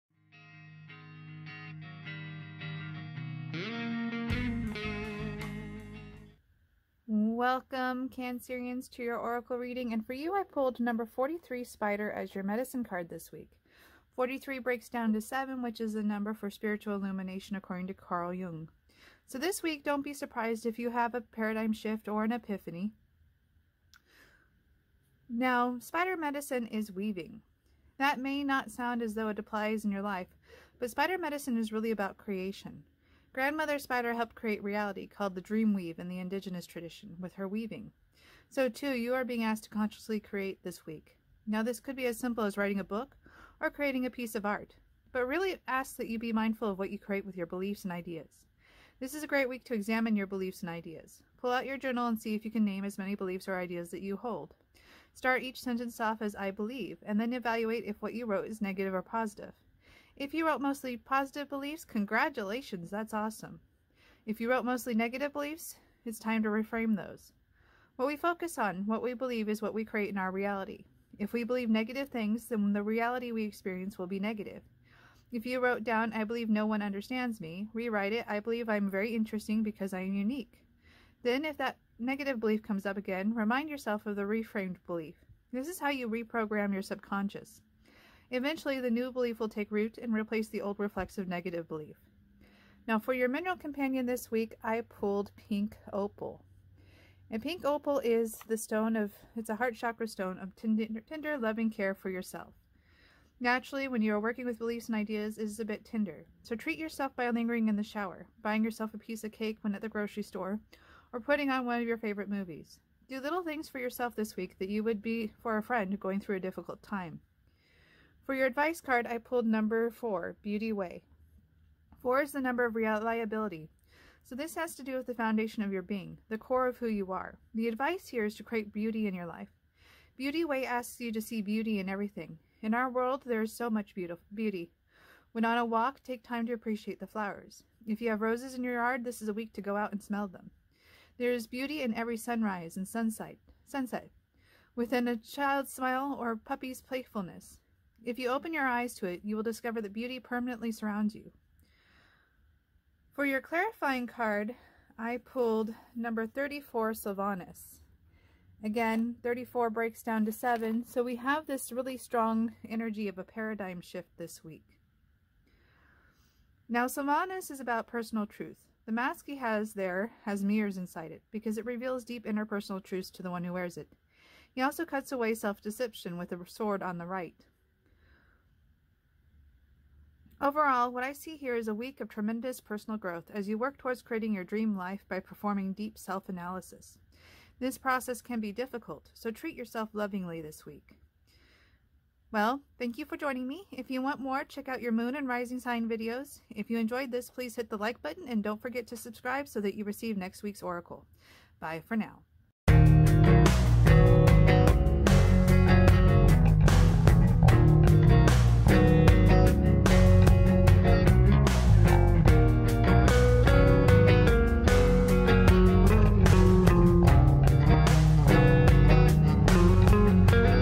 Cancer Weekly Oracle Reading- Beauty Everywhere
I am using three different oracle card decks:

Medicene Cards (Jamie Sams and David Carson)

Mystical Shaman Oracle (Alberto Villoldo, Collette Baron-Reid, and Marcela Lobos)

The Faeries Oracle (Brian Fround and Jessica Macbeth)

You can also buy my book The Purpose of Life: A Scientific and Magical Guide To The Age of Aquarius on Amazon